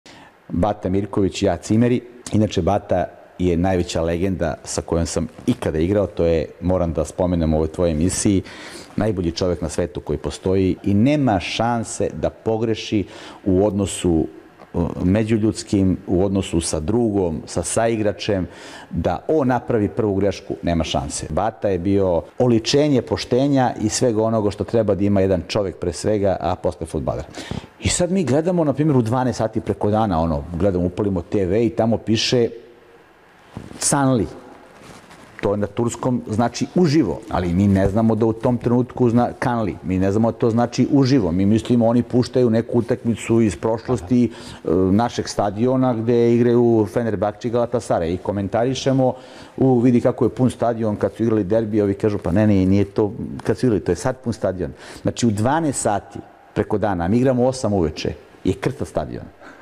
"Bata i ja u šoku, Derbi u 20, a vec oko 12 nema gde igla da padne" - Nikola Lazetić - Mojih TOP 11
U novoj epizodi @mojihtop11, proslavljeni reprezentativac Nikola lazetić priča anegdotu iz vremena kada je sa Baticom Mirkovićem branio boje Fenerbahčeta.